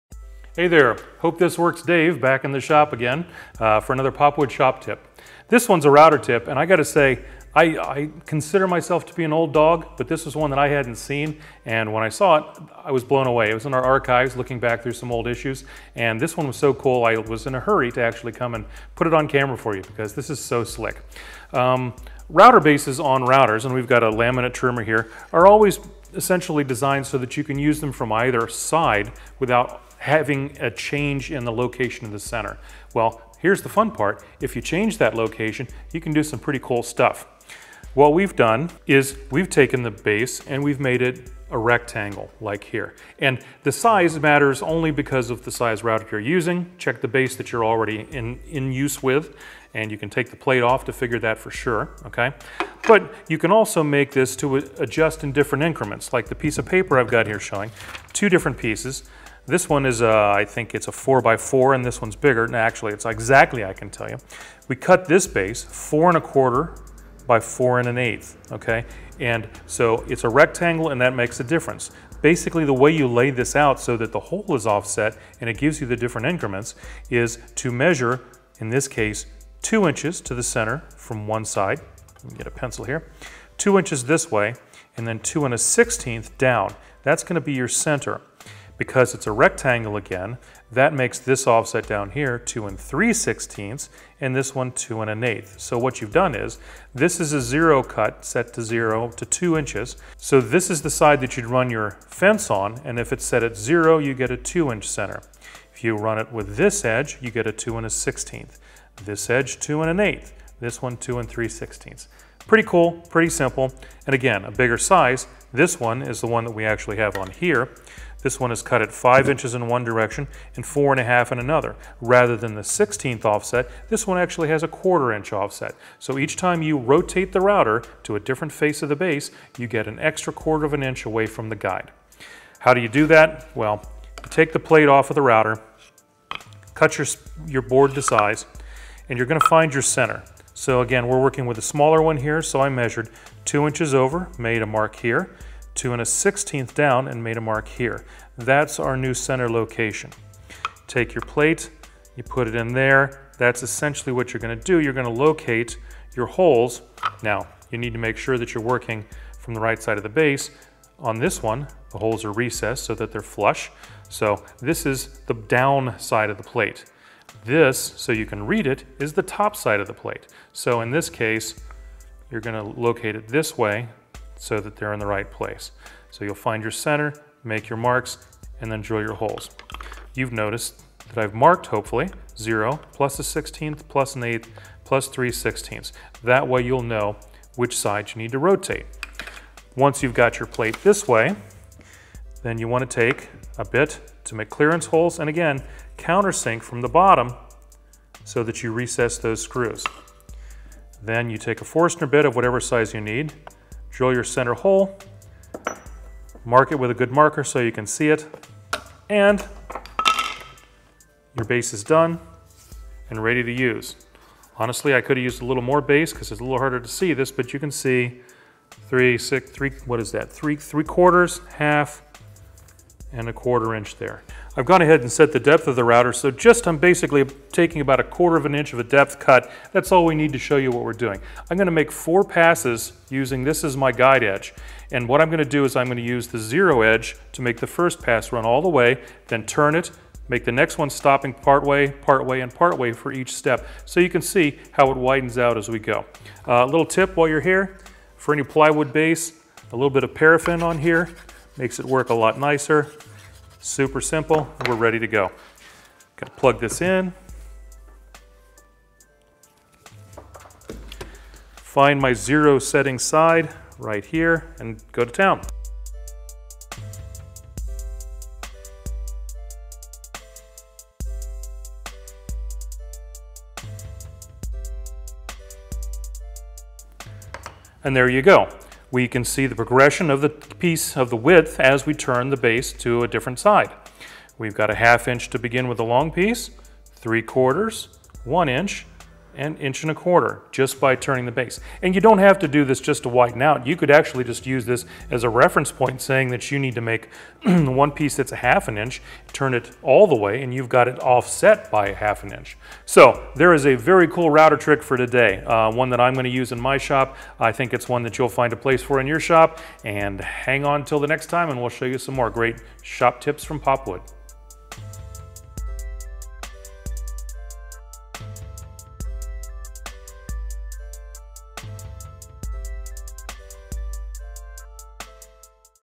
Make a Four-Width Router Plate | Tricks of the Trade
By simply adding a shop-made base to your router you can adjust your cut distance from the fence. A turn (or two, or three) of your router and you've set a new distance. Check it out!